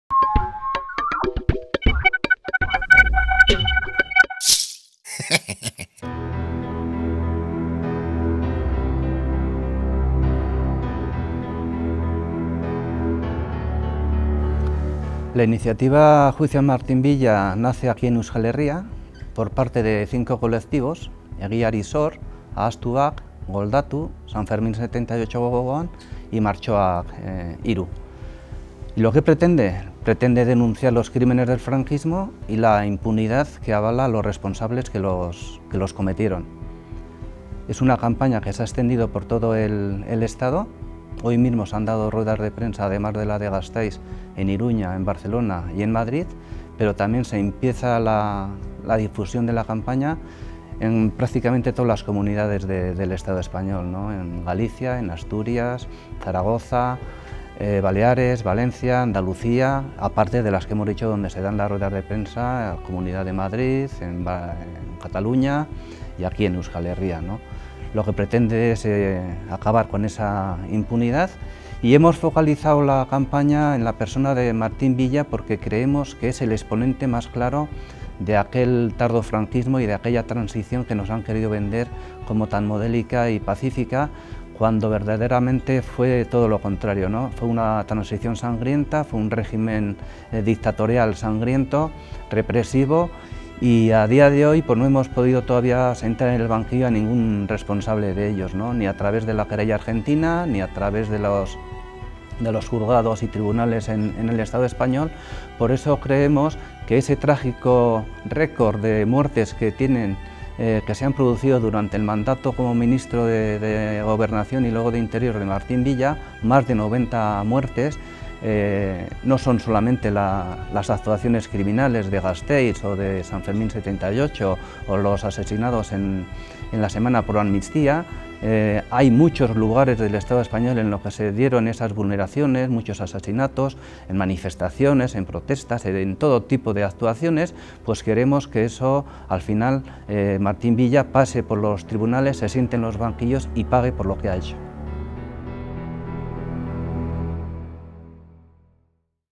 Anuncian el inicio de la campaña 'Juicio a Martín Villa'
Ruedas de prensa simultáneas en diferentes lugares del Estado español han presentado la campaña 'Juicio a Martin Villa' para sentar a uno de los máximos responsables políticos de la masacre del 3 de Marzo, entre otros crímenes, frente a un tribunal.

"La Querella Argentina resulta insuficiente. Sus responsabilidades deben ser depuradas también por los Tribunales del Estado Español". Diferentes colectivos de Gasteiz ya calientan motores de cara al 43 aniversario de la masacre del 3 de Marzo.